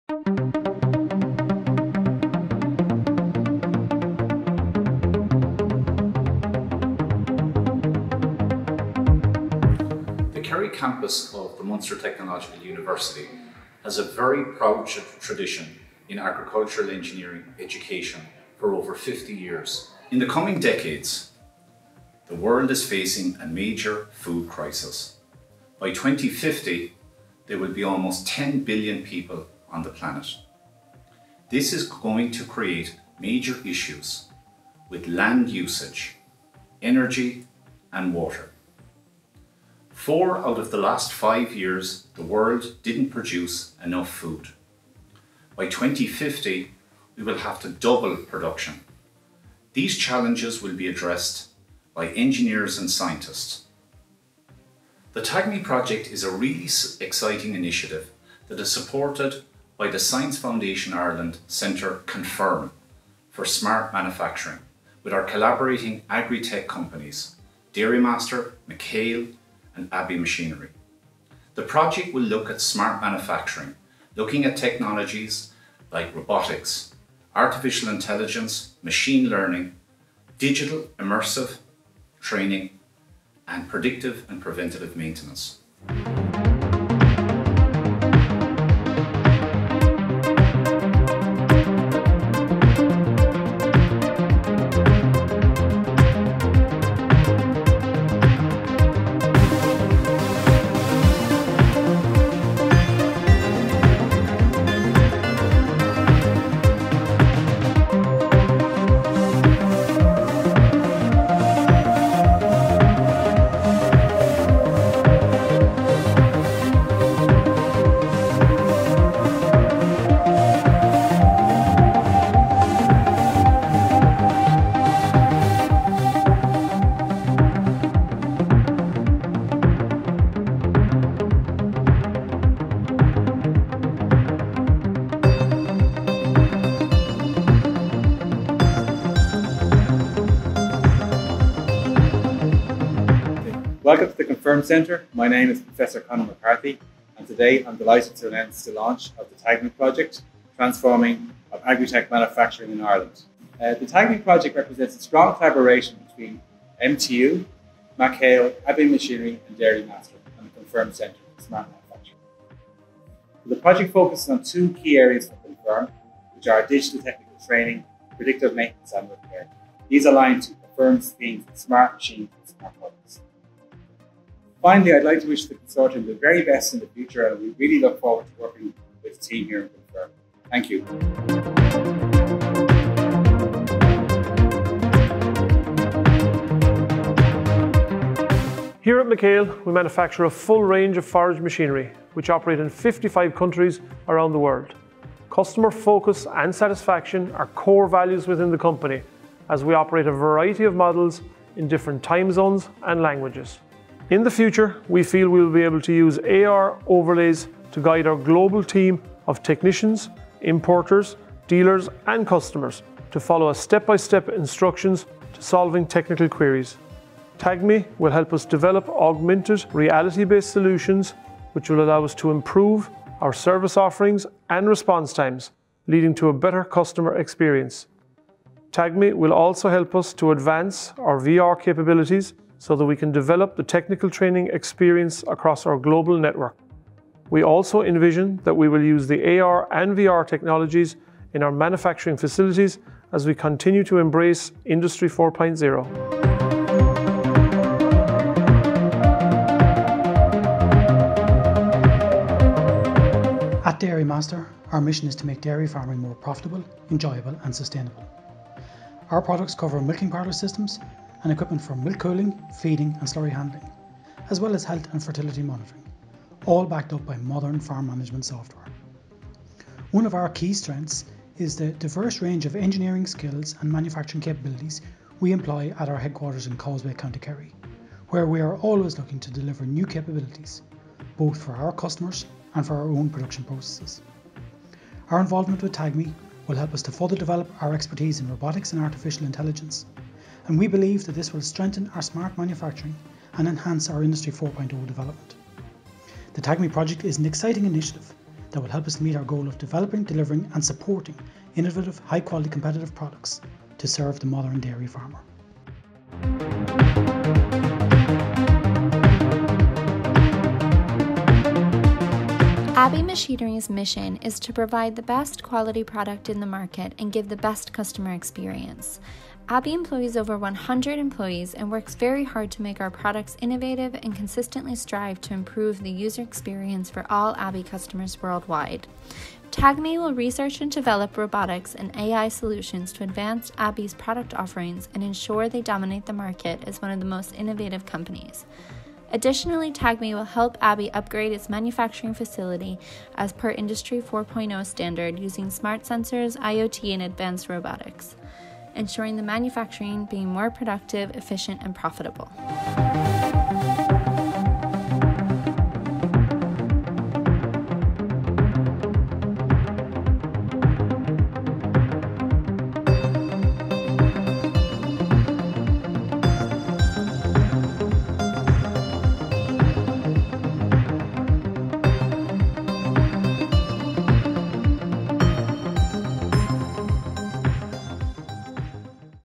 TAgMI Launch
TAgMI (Transforming AgriTech Manufacturing in Ireland) is a significant collaboration between Ireland’s leading AgriTech companies, Science Foundation Ireland CONFIRM Centre for Smart Manufacturing and the IMaR Research Centre. TAgMI will research and develop robotics, AI and immersive digital training solutions to advance current AgriTech products and revolutionise their training and service sector. To achieve the above objectives, the TAgMI project will work with the collaborating AgriTech companies to develop robotics and AI solutions to advance current AgriTech product offerings and develop further innovative assistive technologies for their customers. Additionally, the project will focus on digital training and reimagine and redesign the learning experience and revolutionise the sector using cutting-edge immersive Virtual Realty (VR) and Augmented Realty (AR) technologies and customised E-Learning solutions. The project will generate 11 research positions.